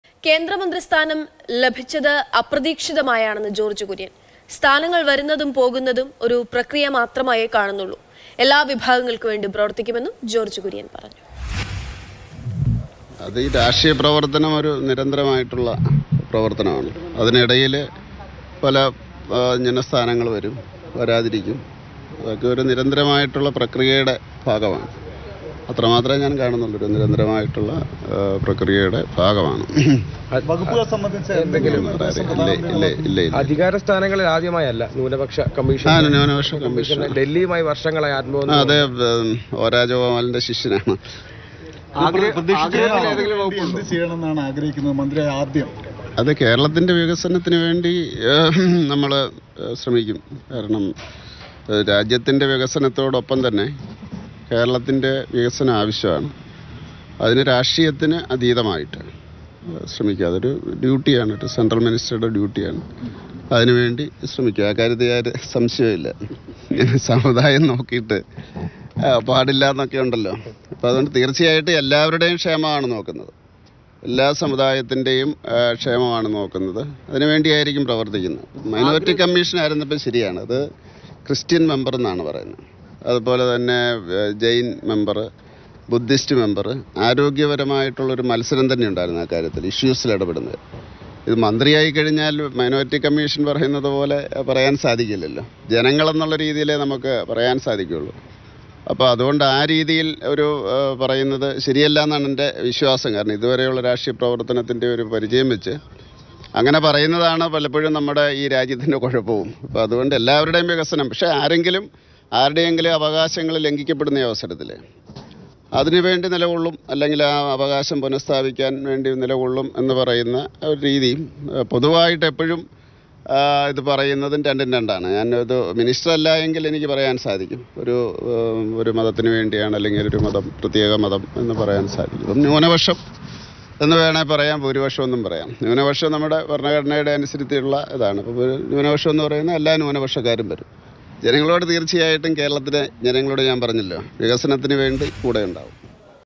'എല്ലാ സമു​ദായത്തിന്റെയും ക്ഷേമമാണ് നോക്കുക; വികസനത്തിന് വേണ്ടി കൂടെയുണ്ടാകും'| George Kurian
എല്ലാ സമു​ദായത്തിന്റെയും ക്ഷേമമാണ് നോക്കുക; വികസനത്തിന് വേണ്ടി കൂടെയുണ്ടാകും -ജോർജ് കുര്യൻ

Happy viewing!